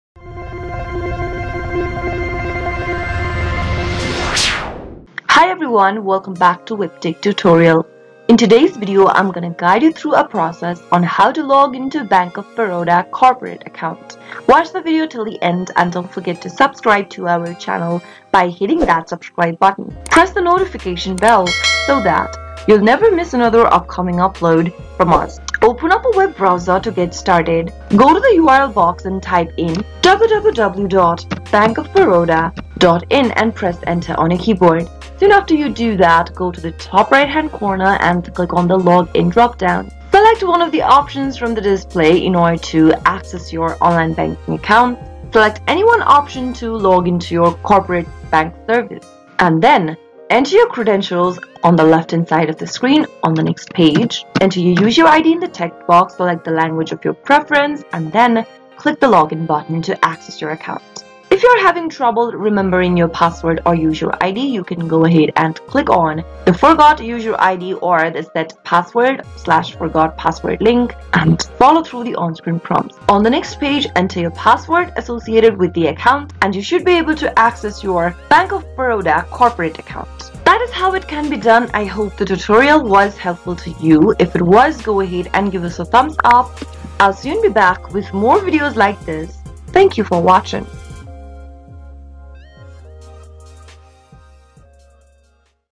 How to Login into Bank of Baroda Corporate Account Online 2023?
Learn how to login to your Bank of Baroda corporate account with this step-by-step guide. This video will show you the entire process from start to finish and help you access your corporate banking services with ease.